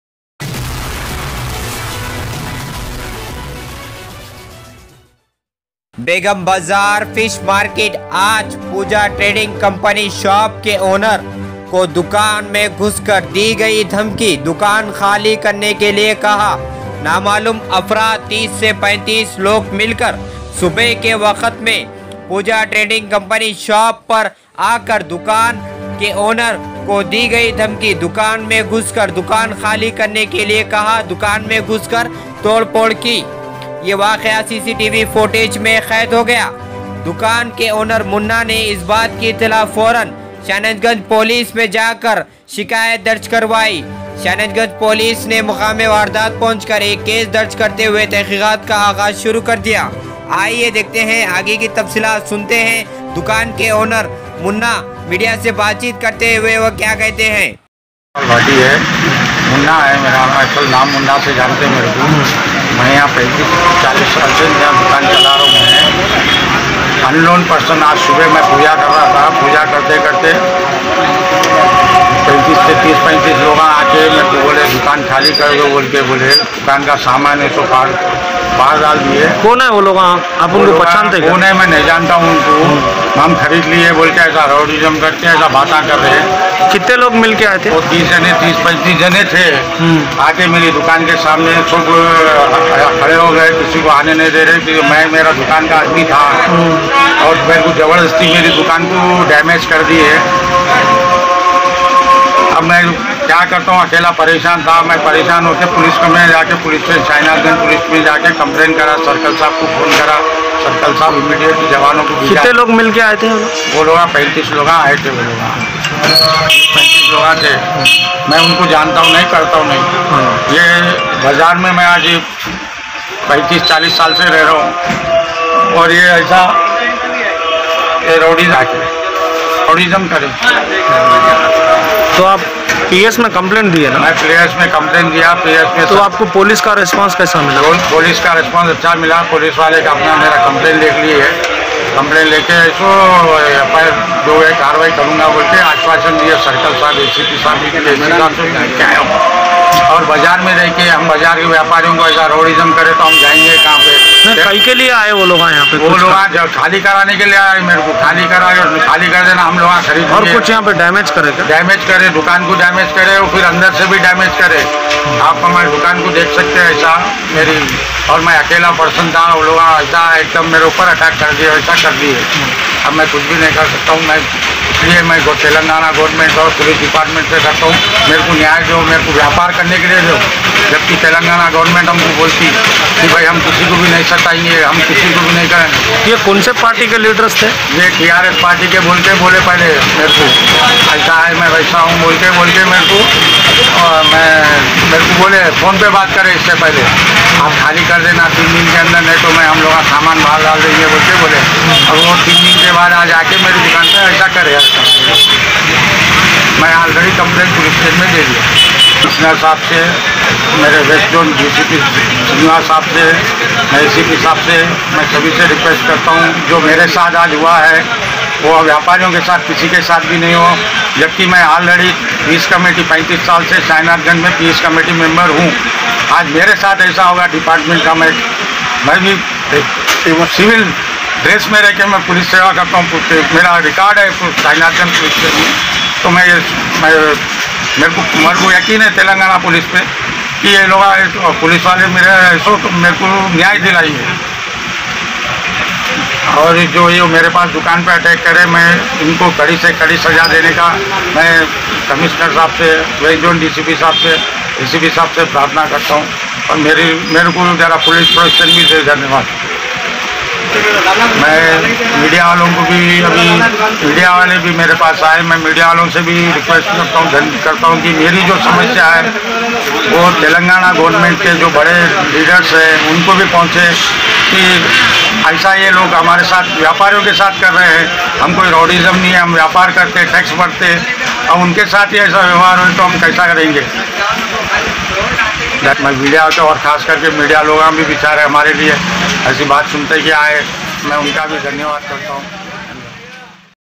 GANG ATTACK 30 SE 35 LOG MILKAR POOJA TRADING SHOP KE OWNER MUNNA KE PER SHAHNATHAJUNG POLICE STATIO
Begum Bazar Fish Market aaj Pooja Trading Company Shop Ke Owner Name Munna Bhati Unknown Person 30 Members Milkar Munna Bhati ke shop per attack Kiya Hai immediately Munna bhathiji ne Shahnathajung police Station me ek complaint Diya Hai Munna bhathiji Ne media se batchit karte hue bataya Hai Ki aur police se request Kiya Hai please take the action breaking news